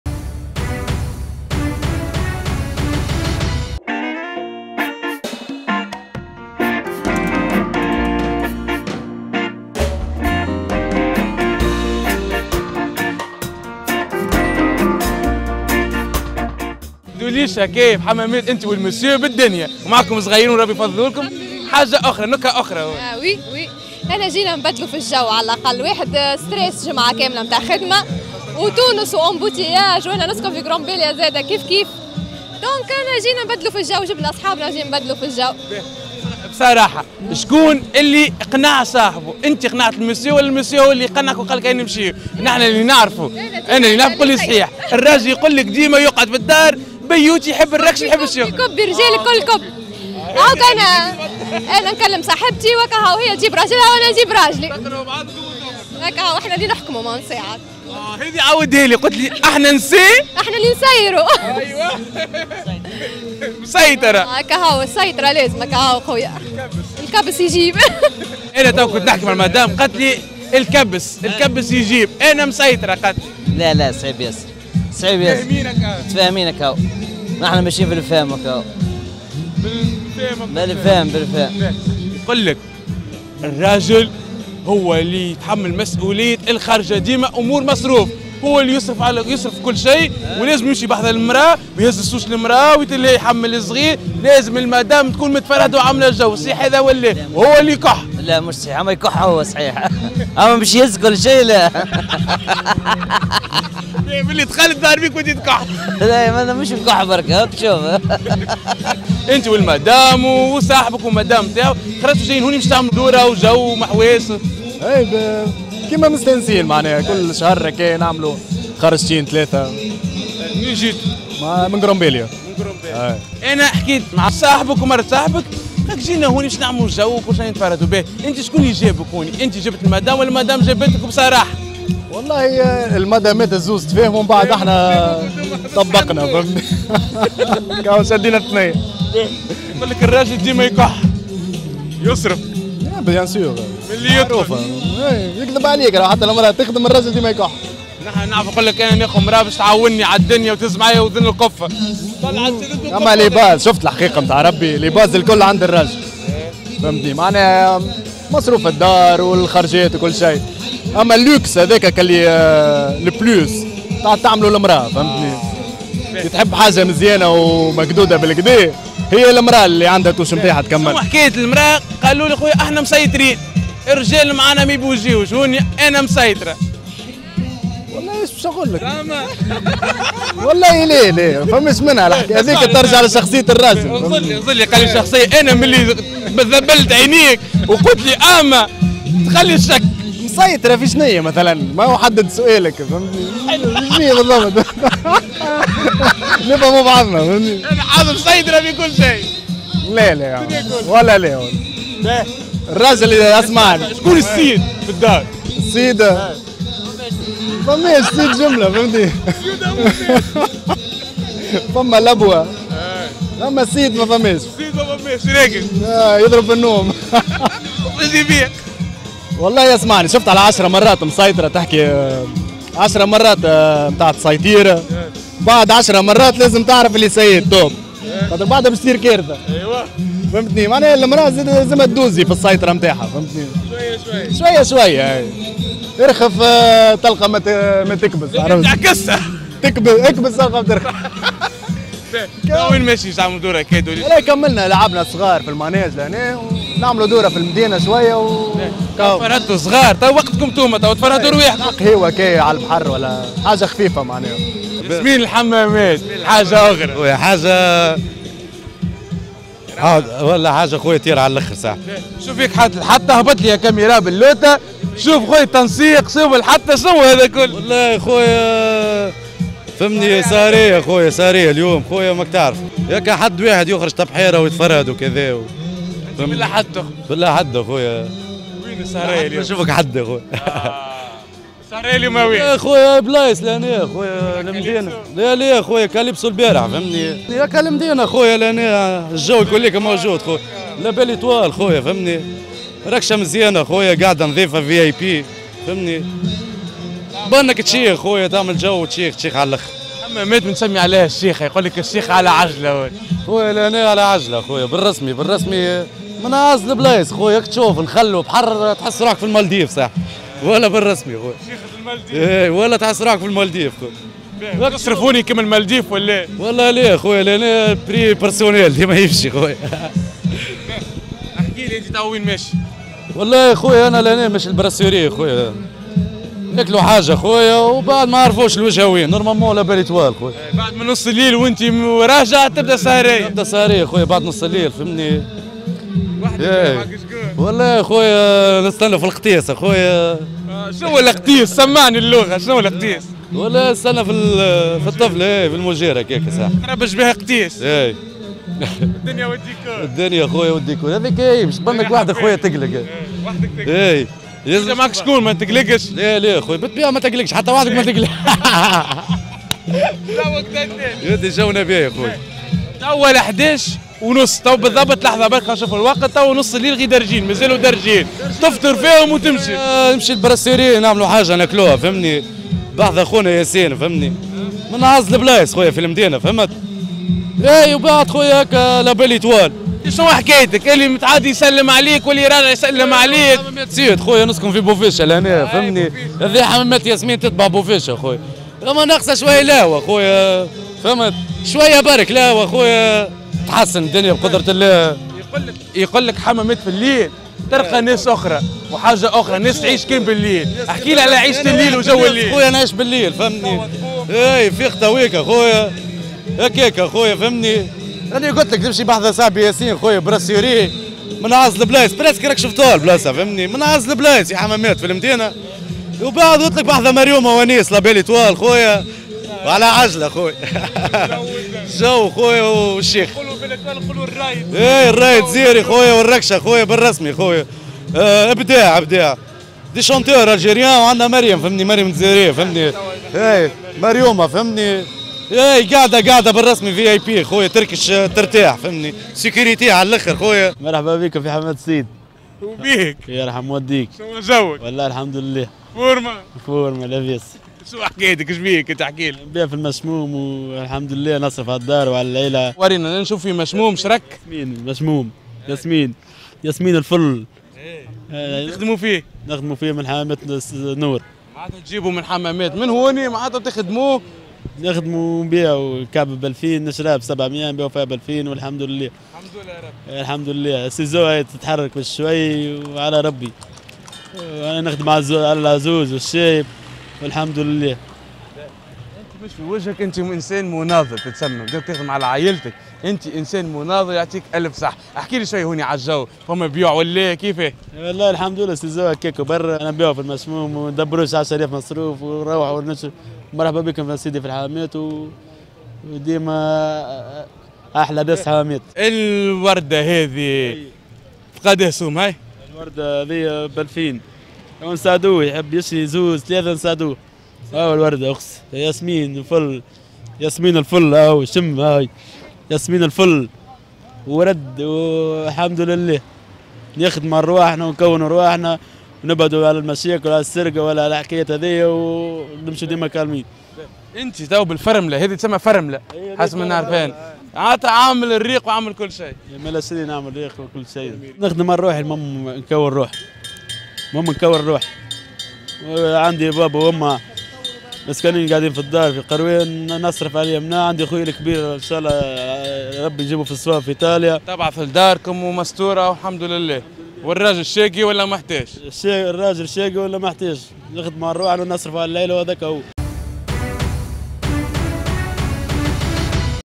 الكوبلوات في الحمامات..الامرأة مسيطرة والراجل ديما يكح هه المادامات يتفاهمو وأحنا نطبقو هه
الكوبلوات في الحمامات..فلوس الخرجة عند الراجل وهو لي ديما يكح
 المادامات يتفاهمو وأحنا نطبقو